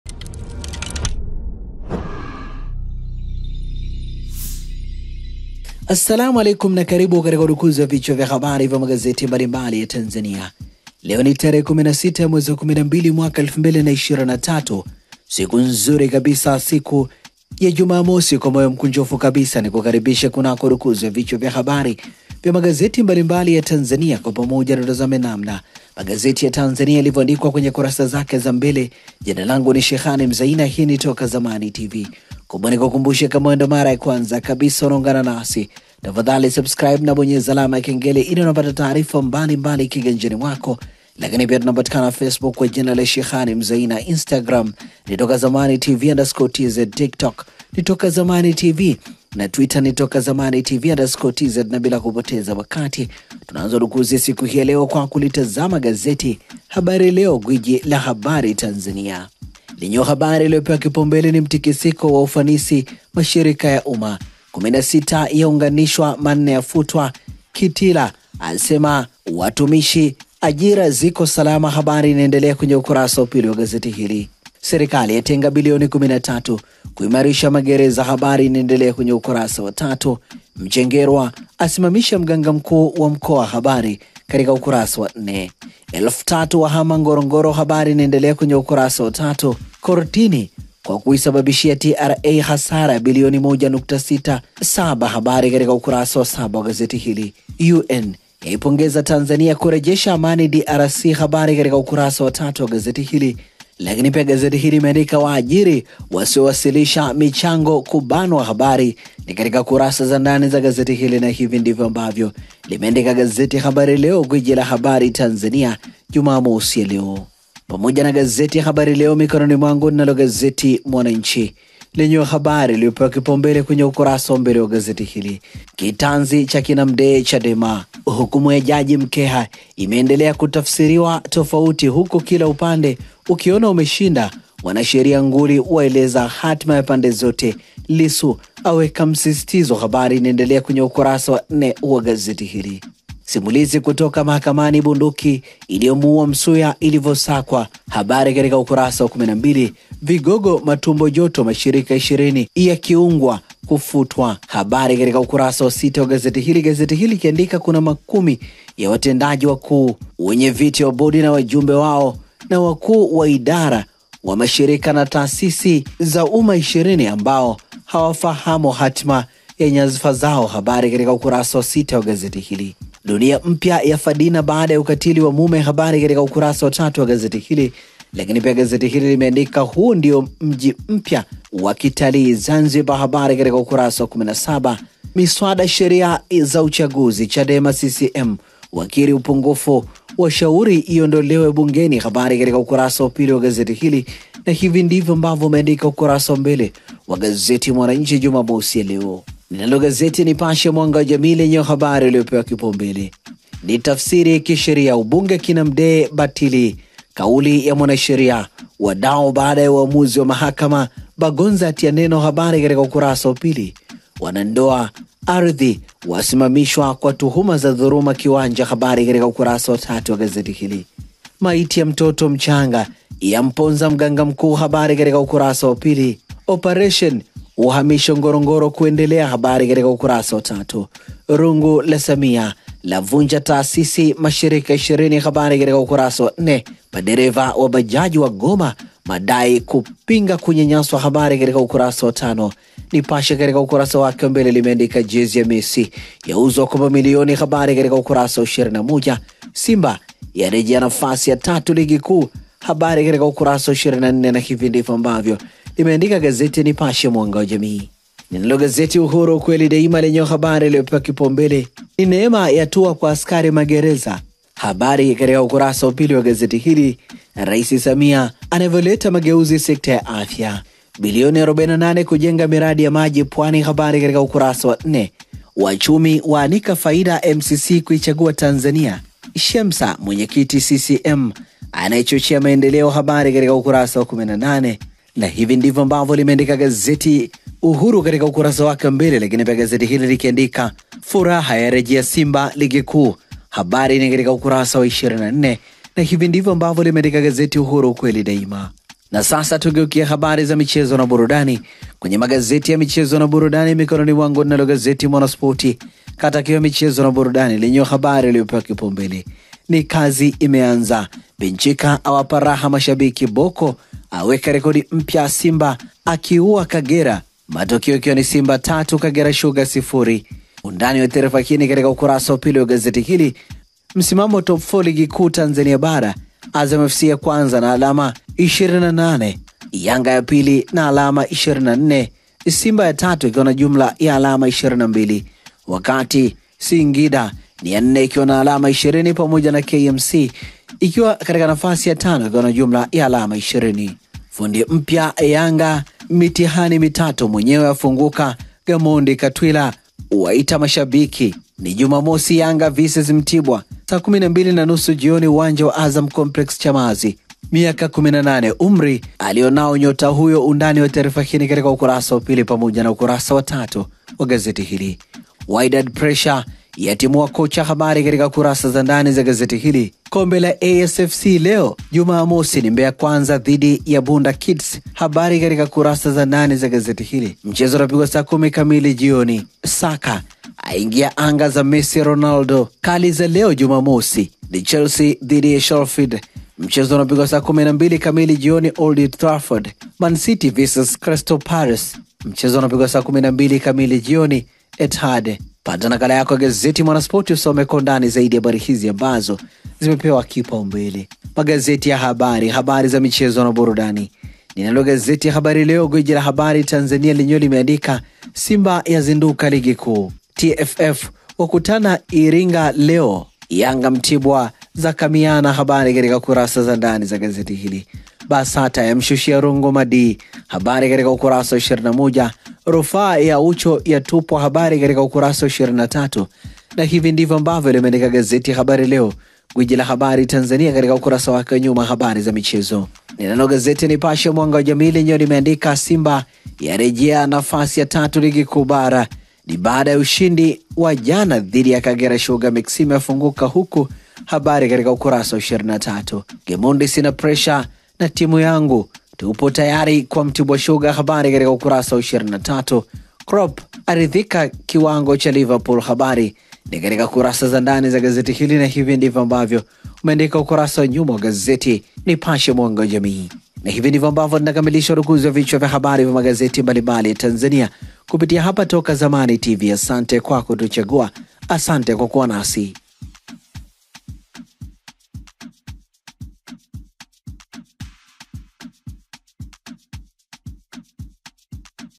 Magazeti ya leo 16/12/23,MCHENGERWA AMSIMAMISHA MGANGA MKUU WA MKOA,KAZI IMEANZA SIMBA,YANGA VS MT..
Magazeti ya leo 16/12/23,MCHENGERWA AMSIMAMISHA MGANGA MKUU WA MKOA,KAZI IMEANZA SIMBA,YANGA VS MTIBWA DIMBANI LEO